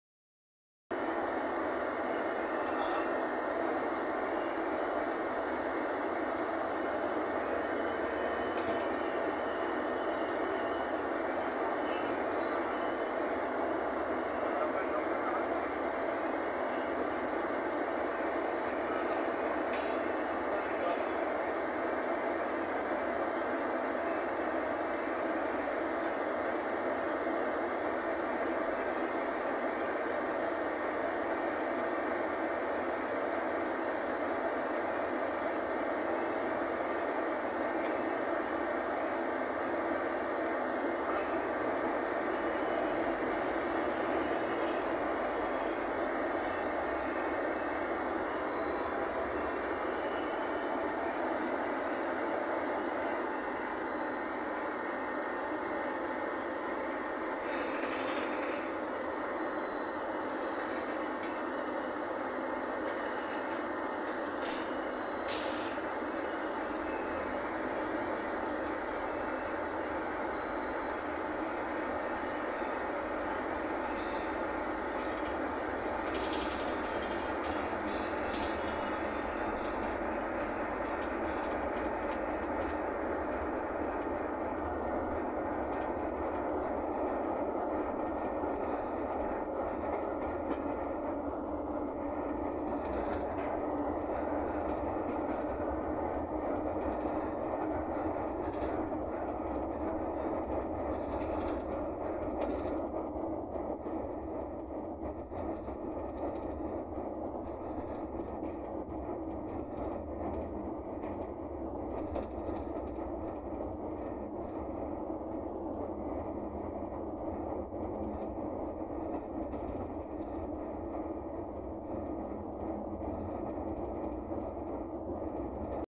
BRT na Pedra de Itaúna !
Novo retorno do BRT Transoeste na Pedra de Itaúna @Mostrandoosonibus